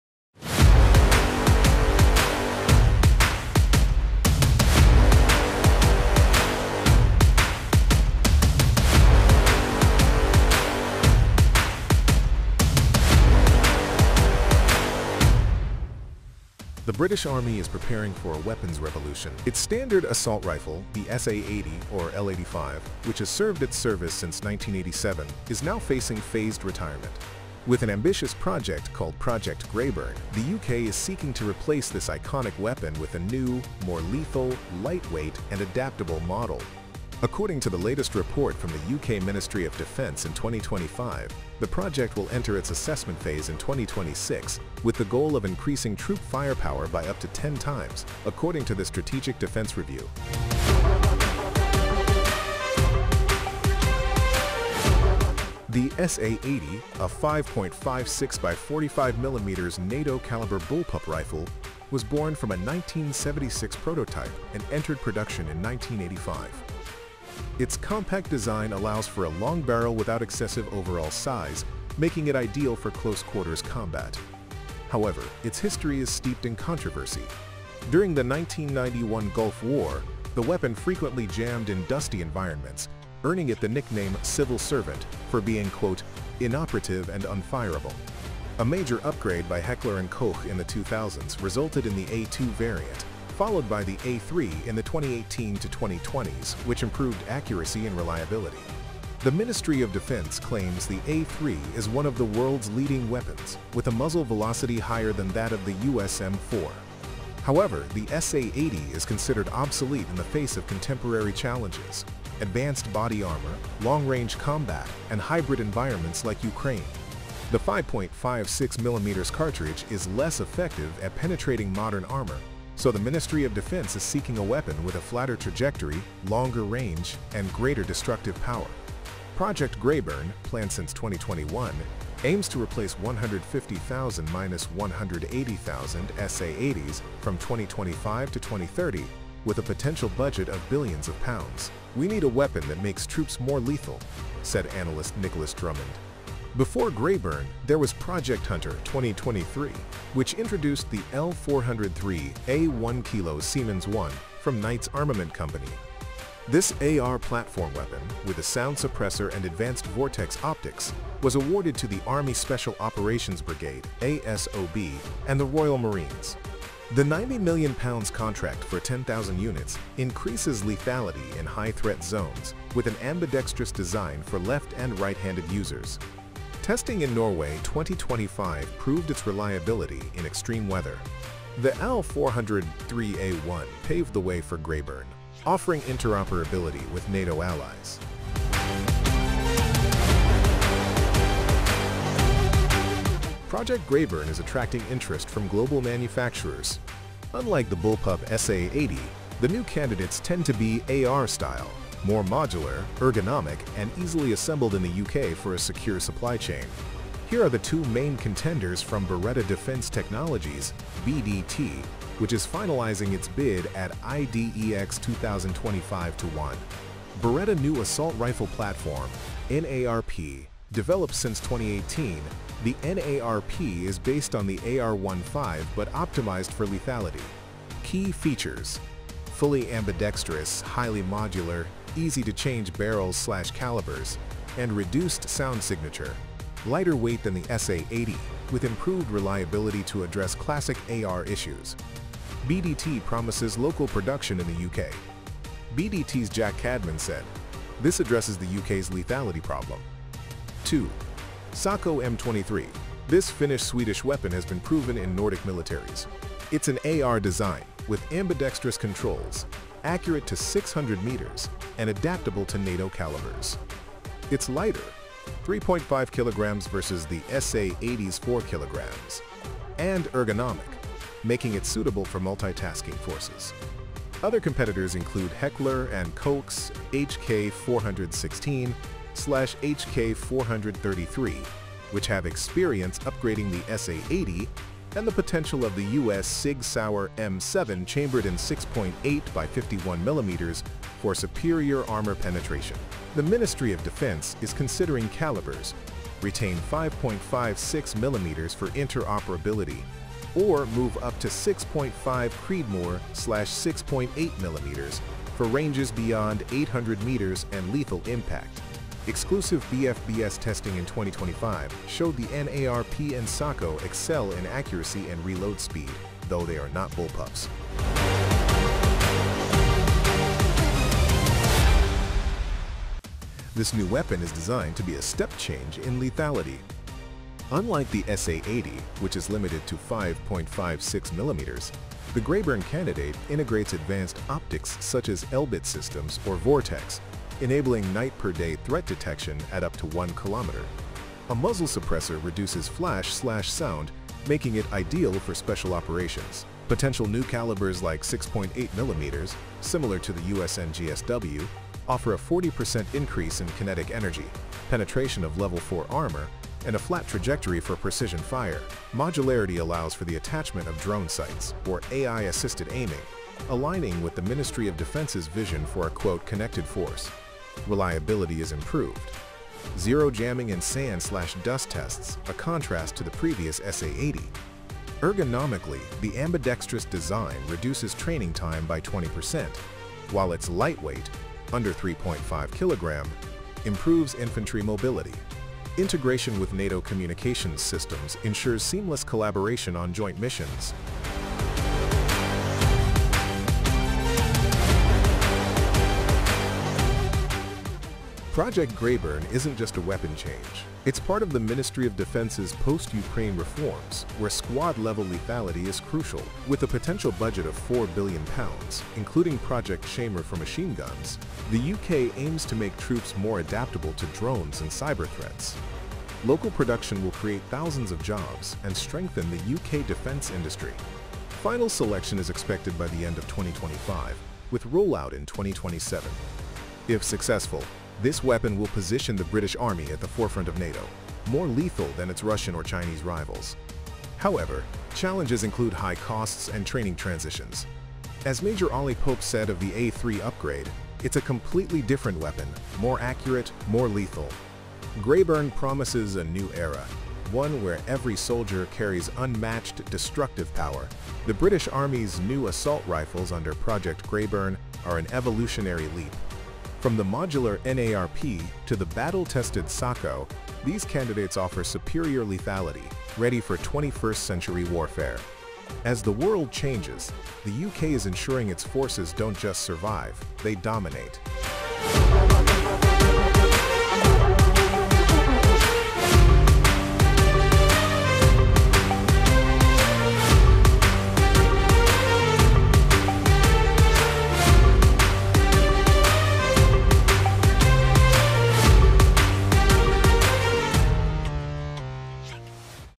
The SA80's replacement! Britain's upcoming new assault rifle is 10x more deadly
Project Grayburn marks a revolution in British infantry weapons: replacing the outdated SA80 with an AR-style assault rifle that is 10x more lethal—a powerful caliber (potential 6.8mm), 800m+ range, Level IV armor penetration, advanced optics, a sound suppressor, and full modularity. With candidates like the Beretta NARP & Sako M23, the British Army is poised to dominate the modern battlefield: from cities to frozen forests. Rollout 2027 will make every soldier a precision killing machine, align with NATO, and strengthen the UK defense industry.